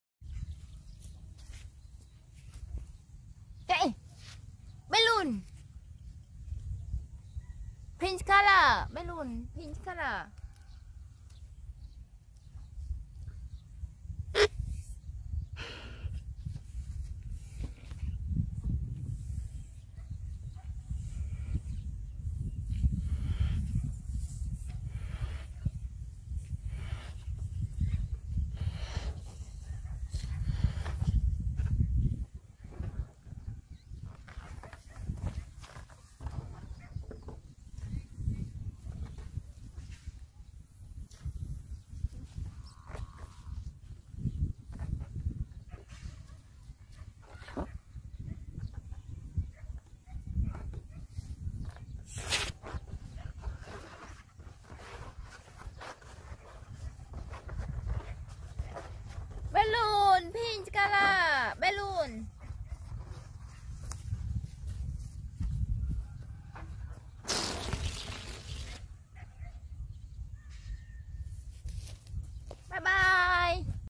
Blow balloons, learn colors, launch balloons popping balloon outdoor fun#42
Welcome to the Oun neth Channel..!learncolors
oun neth channel shows you how to learn colors by blowing balloons.